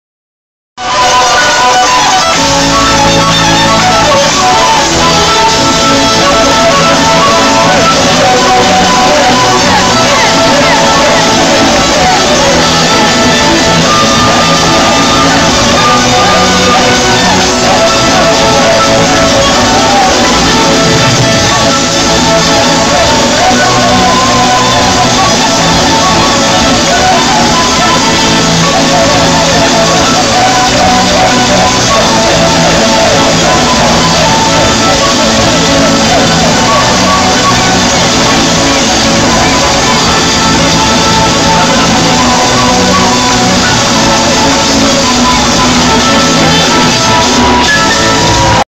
2. MAGO DE OZ. Un solo
Concierto MAGO DE OZ en Chile
teatro caupolican
2011